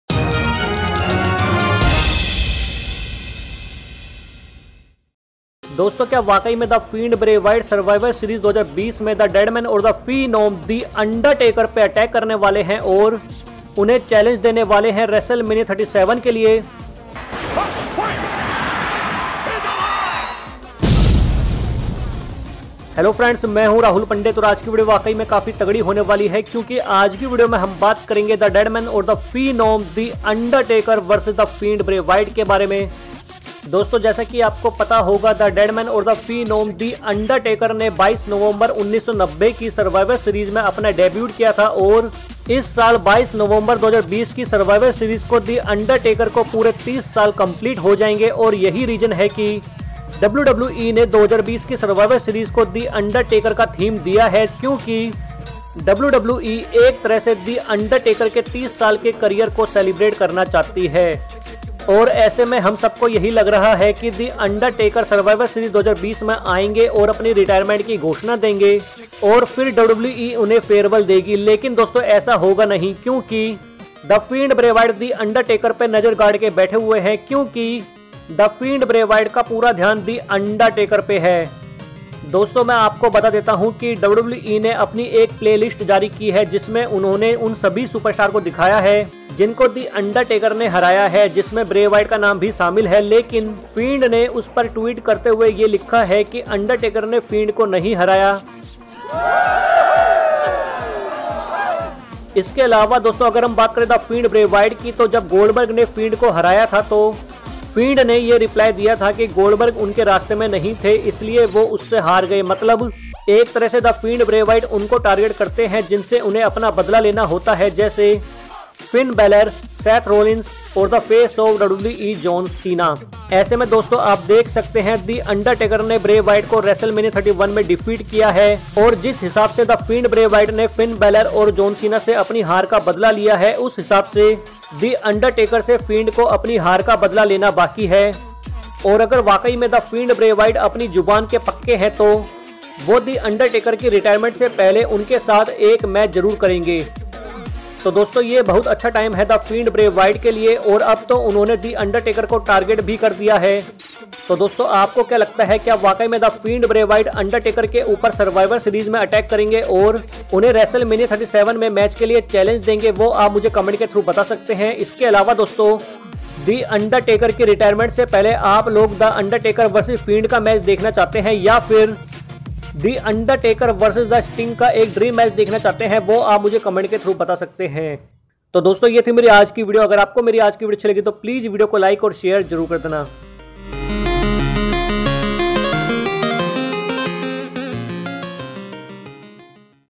The Fiend Attack Undertaker Survivor Series 2020 & Challenge Wrestlemania 37 Highlights, Predictions
Dosto इस Video में Survivor Series 2020 में The Undertaker V/S The Fiend Bray Wyatt के Match के बारे में बताया गया हैं !

Dosto Must Watch My All Videos:-